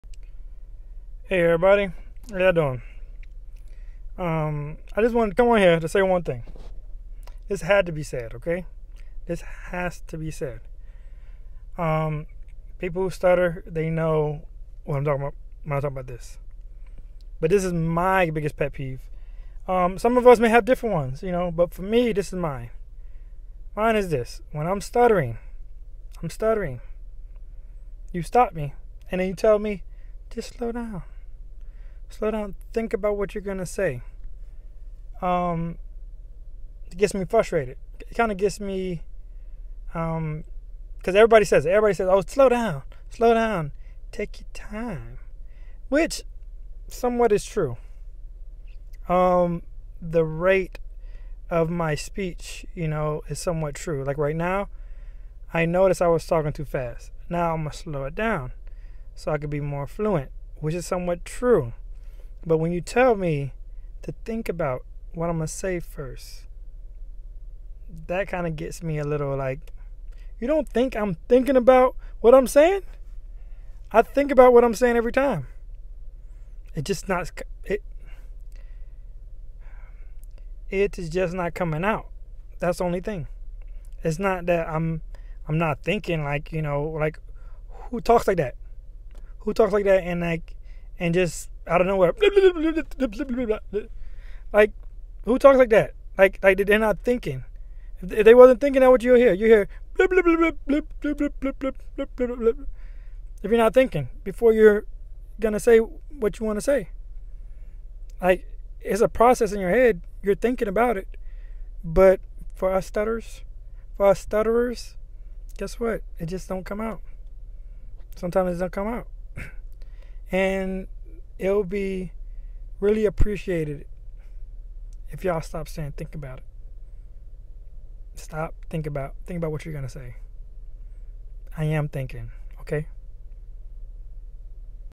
Please Stop telling us this
One of my biggest pet peeves that makes my stuttering get worse